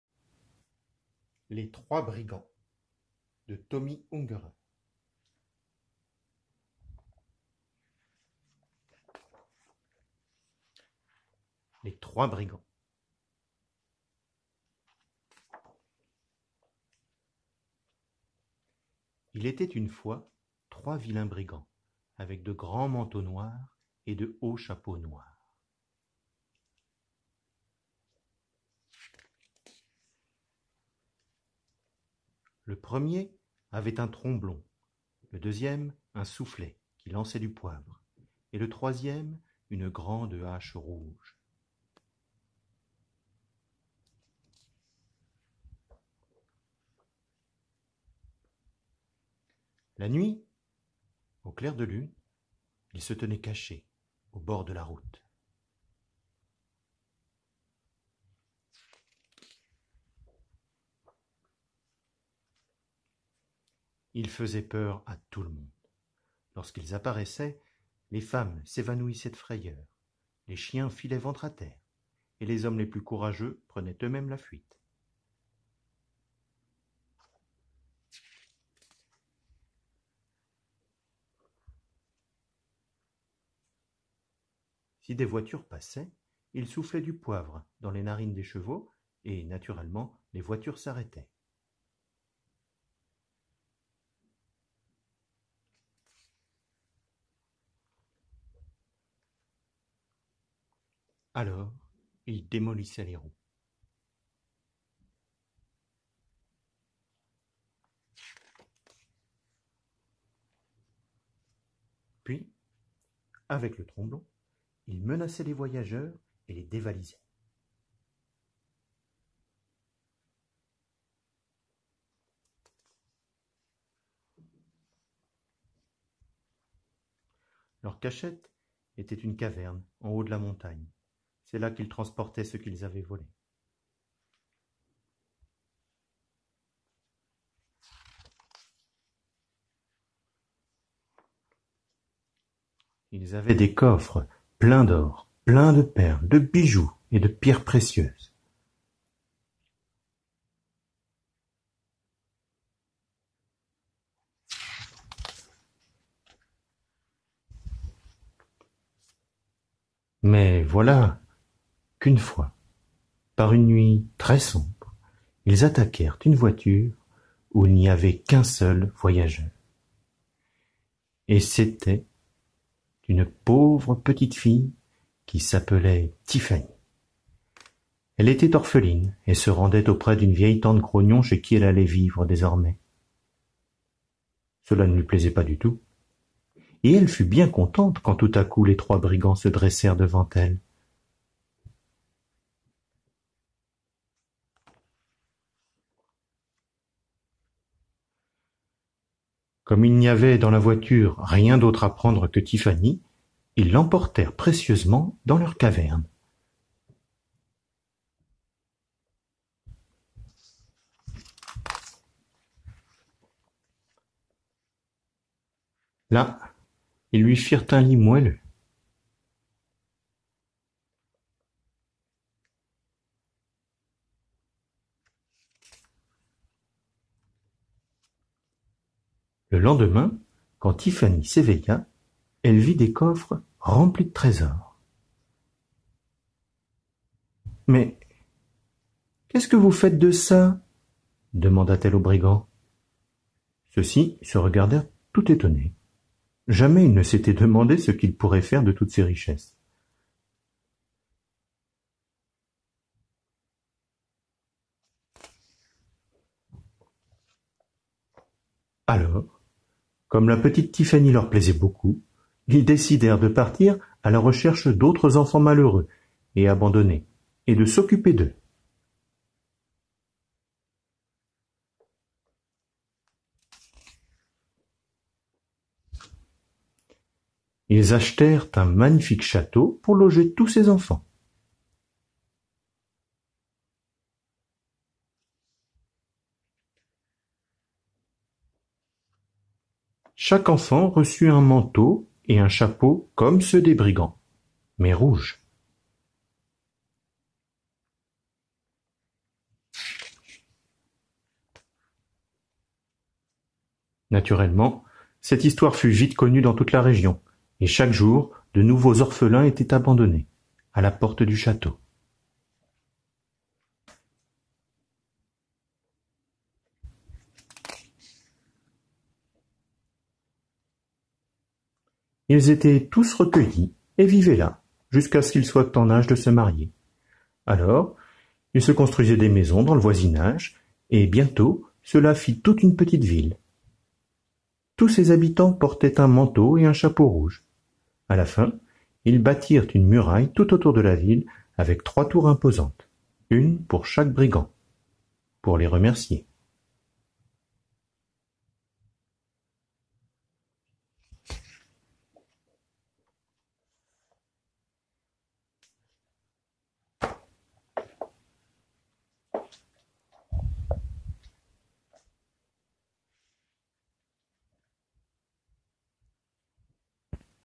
Lecture "Les Trois Brigands"
Tomi Ungerer, Les Trois Brigands. Lecture de l'album.